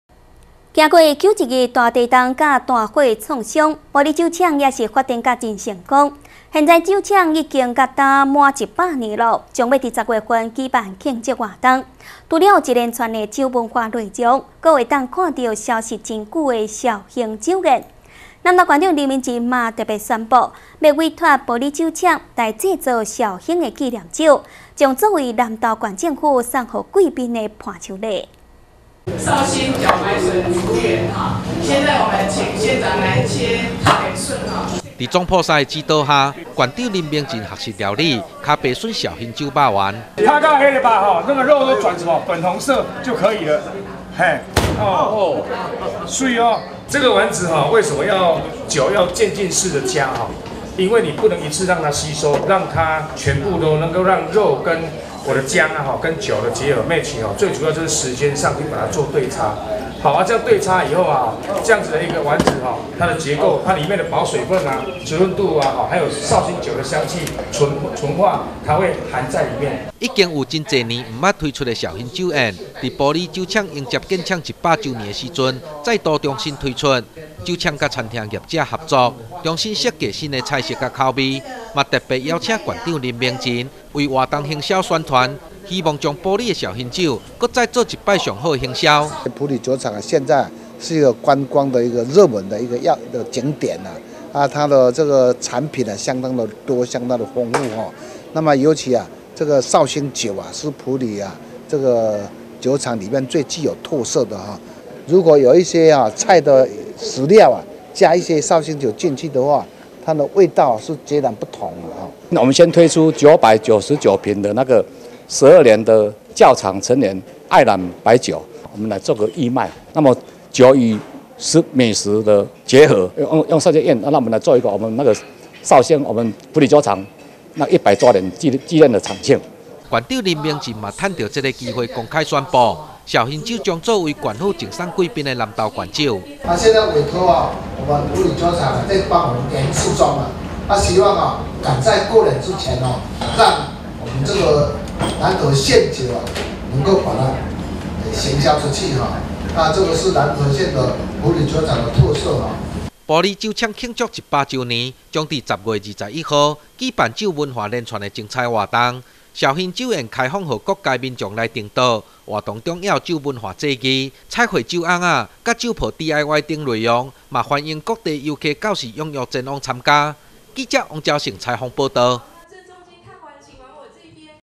南投新聞 埔里酒廠慶百年再推紹興酒宴
挺過921大地震重創，又歷經祝融肆虐，埔里酒廠依舊屹立不搖，創廠至今屆滿百週年，埔里酒廠，將於十月舉辦百周年慶，除了一連串酒文化活動之外，消失已久的紹興酒宴再度重現江湖，邀請各界民眾一起來共享埔里的酒文化饗宴，南投縣長林明溱也特別宣佈，委請埔里酒廠製作紹興酒紀念酒，將做為南投縣政府贈送貴賓的南投縣酒。